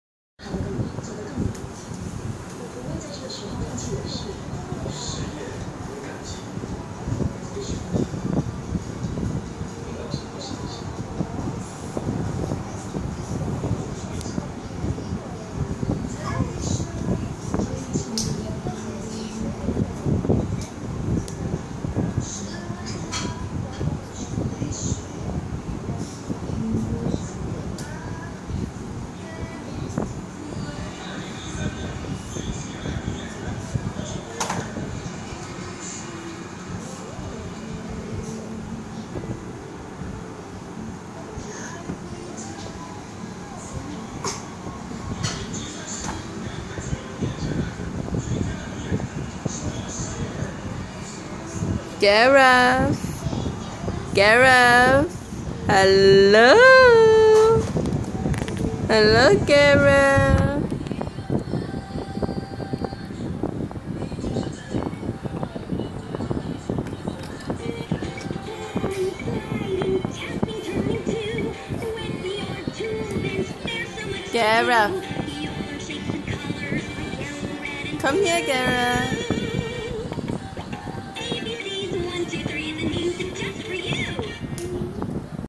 Gareth is crawling fast
At 7 month old